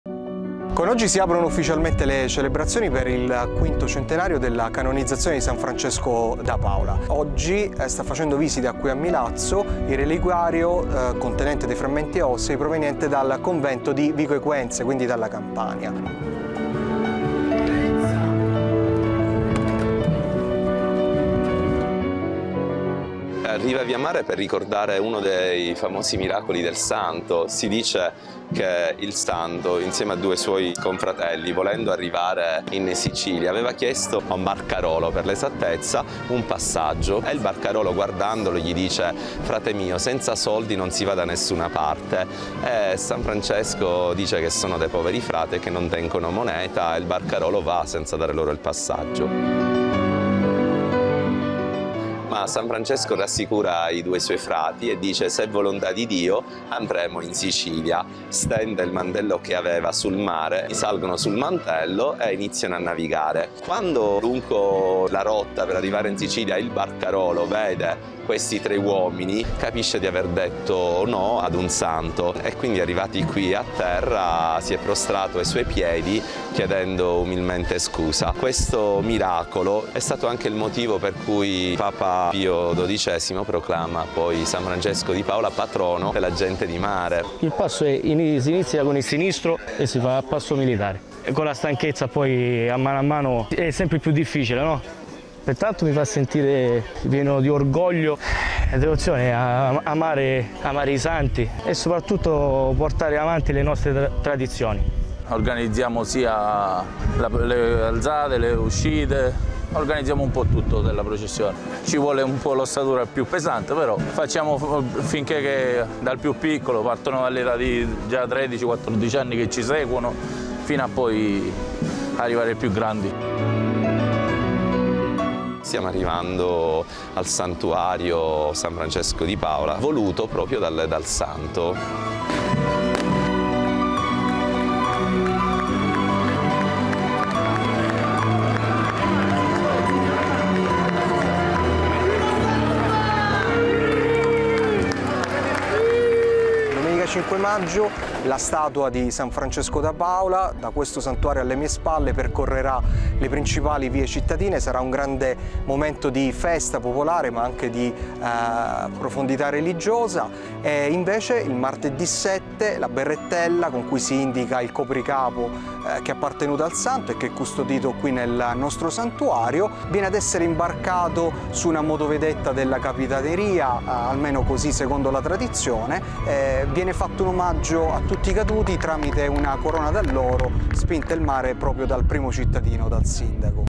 Milazzo, accoglienza San Francesco di Paola
A Milazzo, in provincia di Messina, si è svolta la cerimonia per l’accoglienza del reliquiario di San Francesco di Paola proveniente da Napoli. In Sicilia, c’è un gran fermento per le tante iniziative per celebrare il V Centenario della Canonizzazione di San Francesco di Paola, il Santo che è arrivato dal mare.